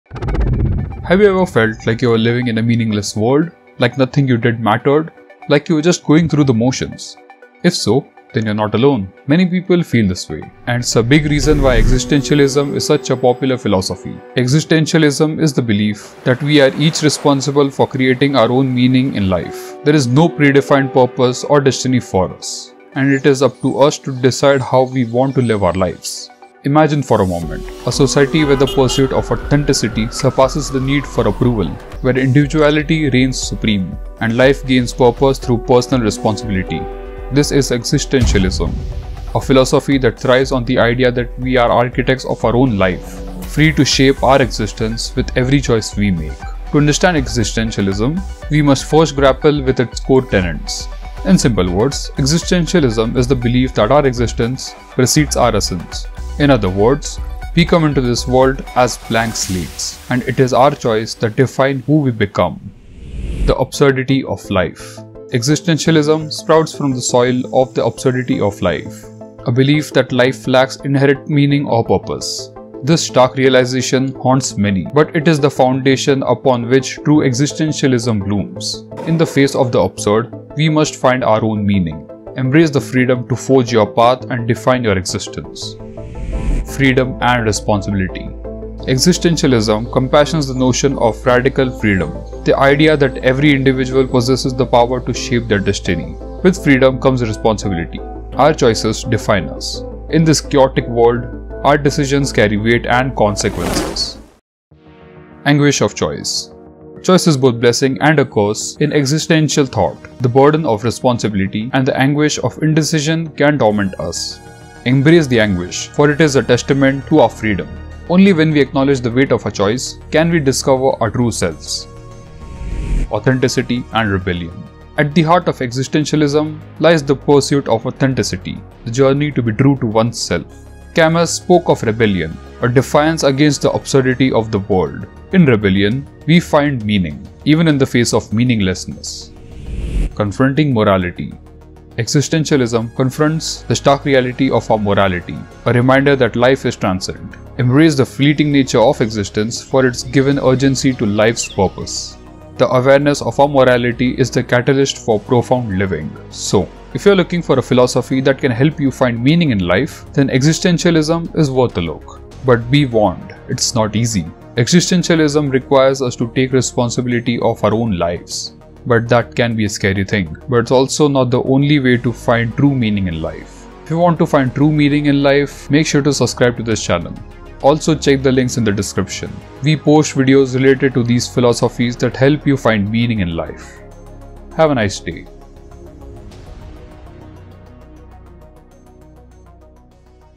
AVOID Getting Caught: The TRAP of Existentialism
What is Existentialism? Existentialism is a philosophy that explores the problem of human existence, with an emphasis on the individual who starts in an apparently meaningless world, and who seeks to create meaning in a world without inherent meaning.

Philosophers have been dealing with the problem of meaninglessness for centuries. Their ideas about life (or how to live well in a meaningless universe) could be valuable to those struggling with a lack of meaning in their lives. Can we live well as humans when life has no inherent purpose? According to several philosophers: yes, we can. And, according to some, we should. After all, if life loses meaning, there’s still life.

Video: Avoid Getting Caught: The Trap of Existentialism